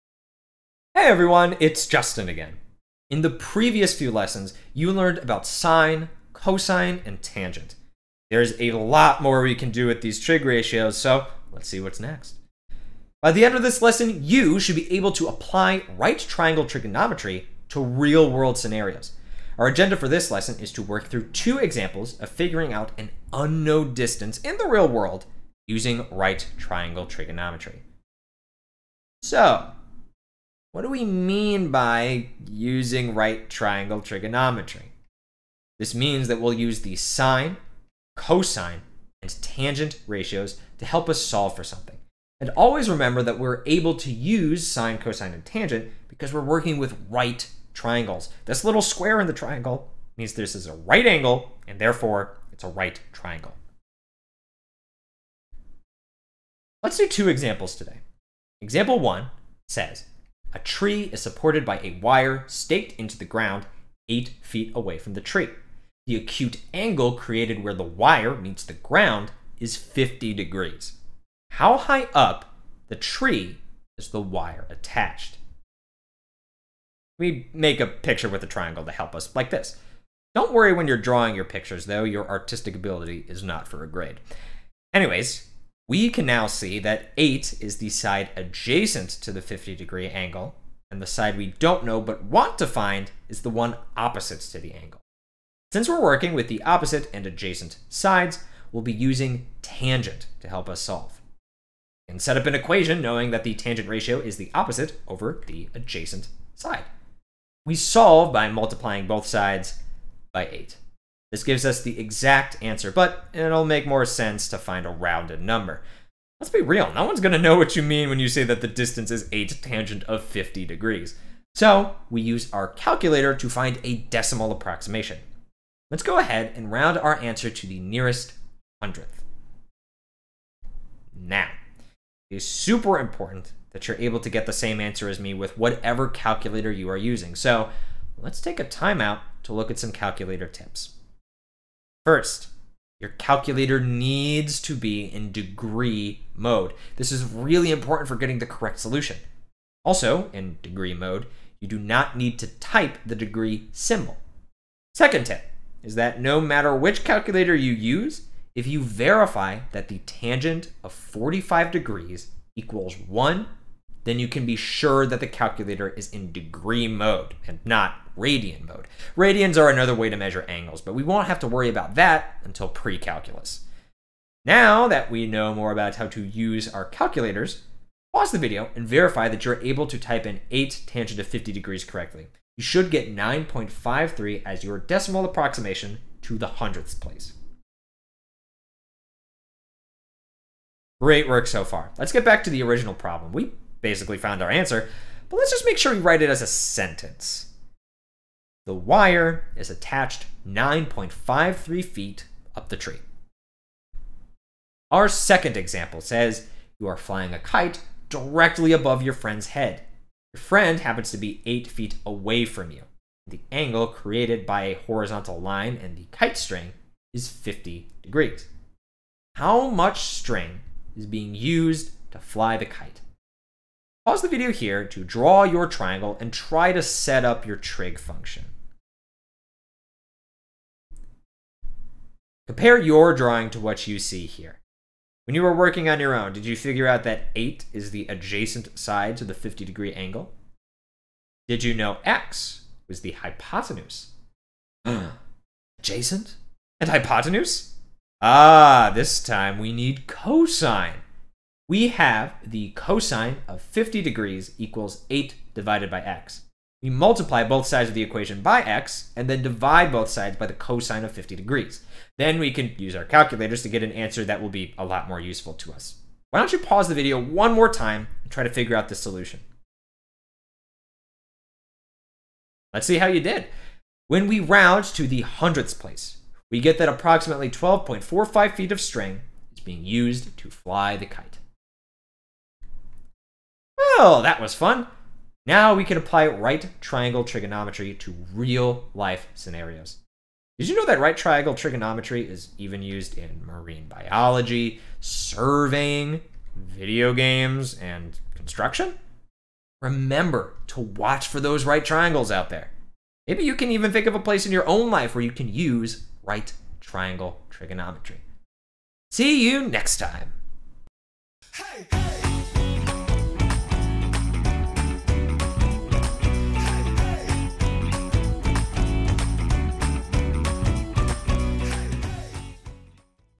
Applications of Right Triangle Trigonometry - Solving Real-World Problems
Want to solve triangles using real-world clues? In this Algebra 2 lesson, students will use right triangle trigonometry in practical applications. This lesson is from MiaPrep’s Algebra 2 course.